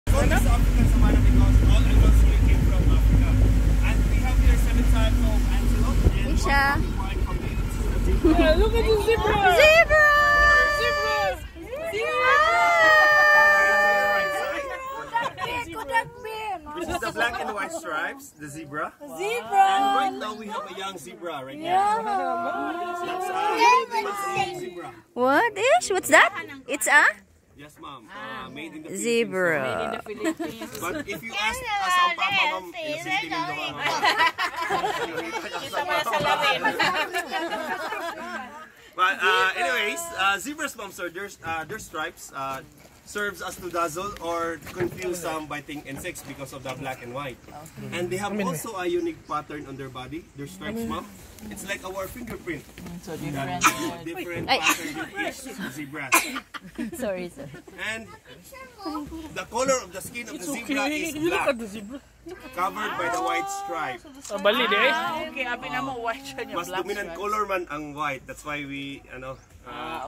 Zebra Encounter at Cebu Safari and Adventure Park
Come and Visit Cebu Safari and Adventure Park in Cebu and see these Creatures closely..They are real!!!